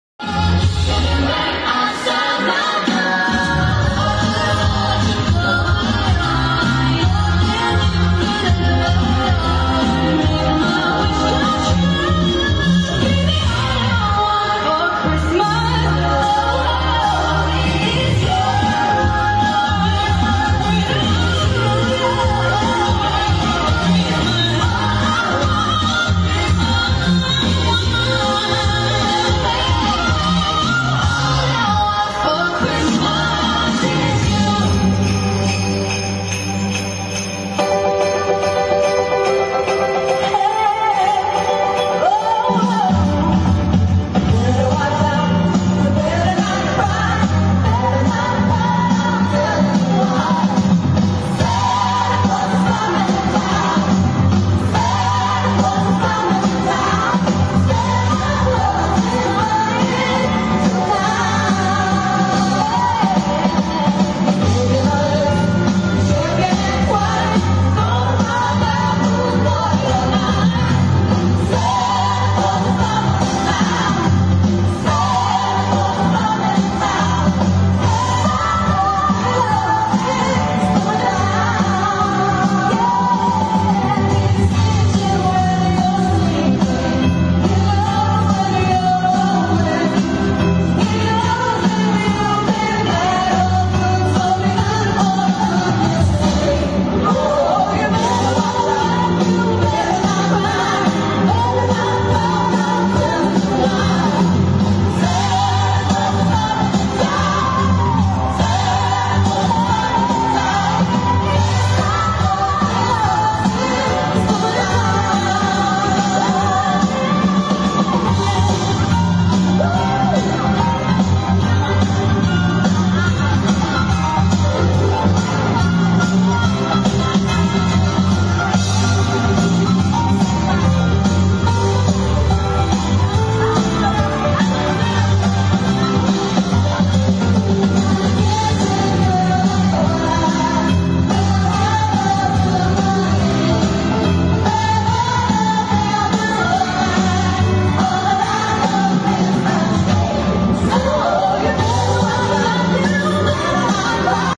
Napoli - Clemente: Luci di Natale spettacolo (09.12.21)
- Napoli - Clemente: Luci di Natale spettacolo
❤️Palazzo Fuga da stasera fino al 12 dicembre, dalle 18 alle 24 ogni ora si potrà assistere allo spettacolo;
❤️Maschio Angioino, dal 23 al 27 dicembre (escluso il 24) ogni ora dalle 17 alle 24;
❤️Castel dell’Ovo, dal 29 dicembre al 6 gennaio (escluso il 31 dicembre) sempre ogni ora dalle 17 alle 24.